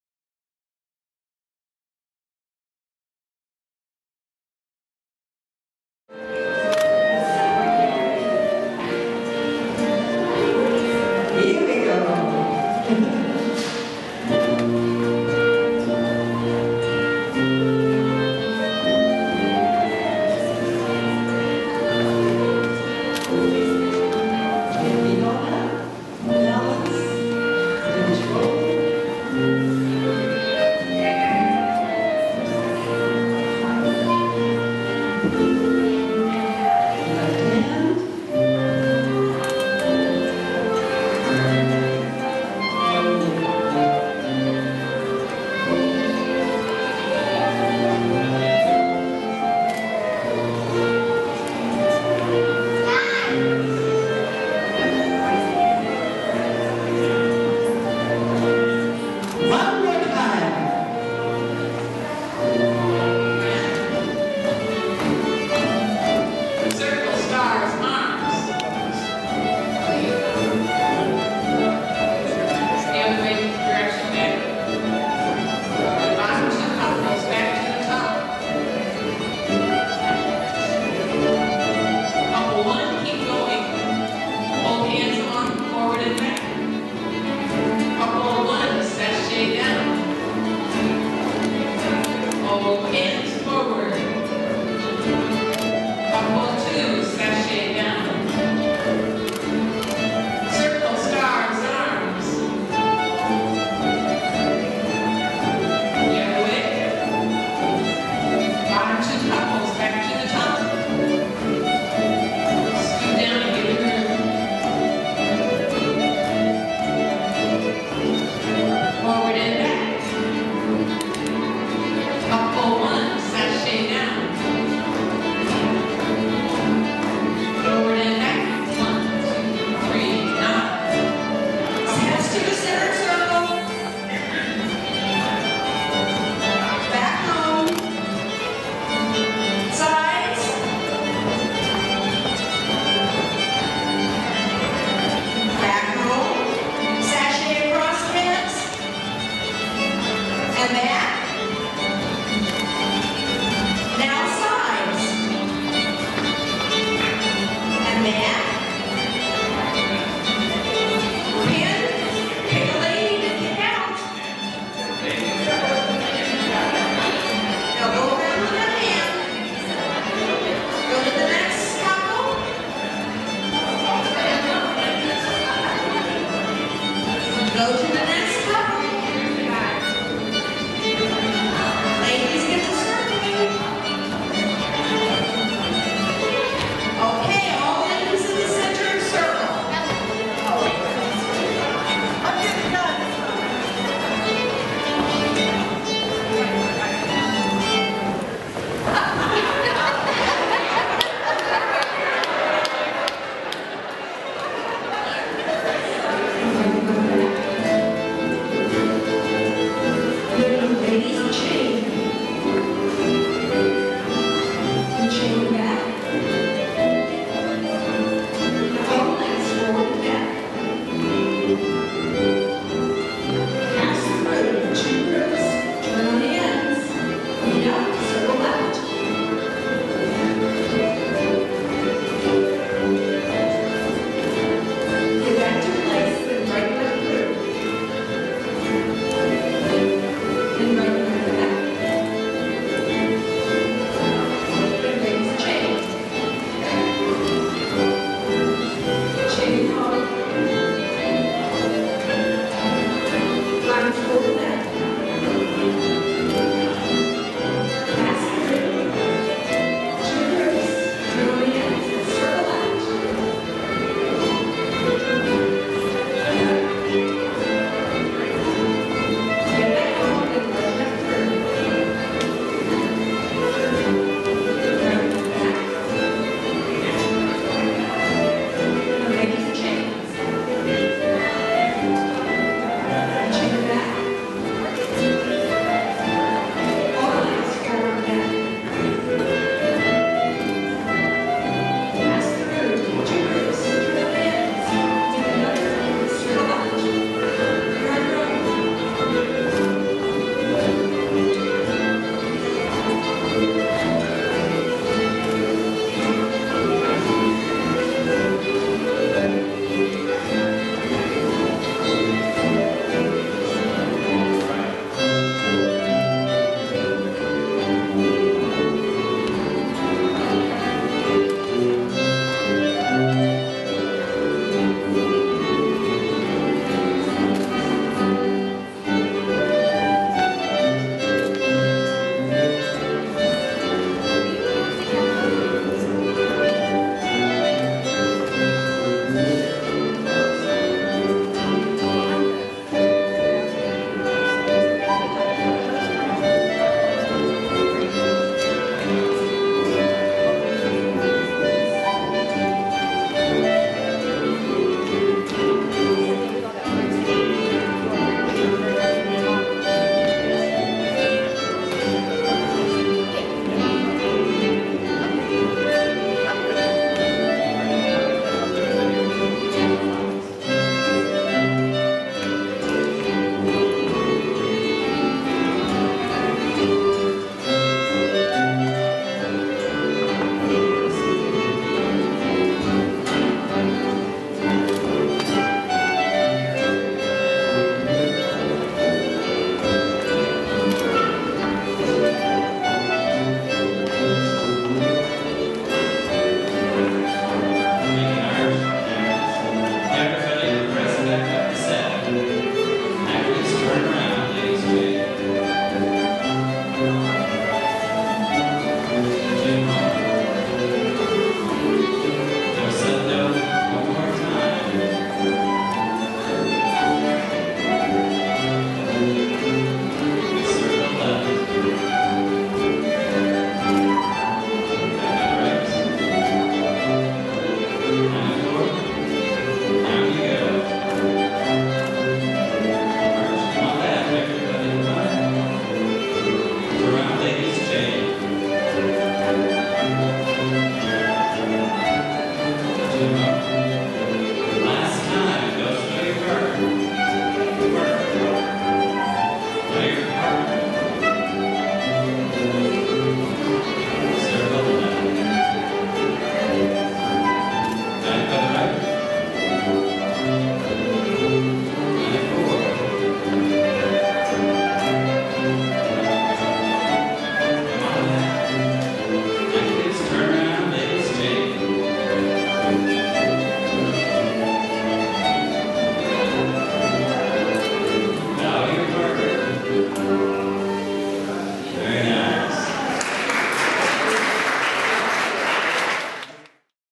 Springfield International Folk Dancers - First Night 2012
Highlights from the Springfield, IL "First Night 2012" Civil War-era ball at the Old State Capitol Historic Site on December 31, 2011. You can learn more about their dance group or their YouTube channel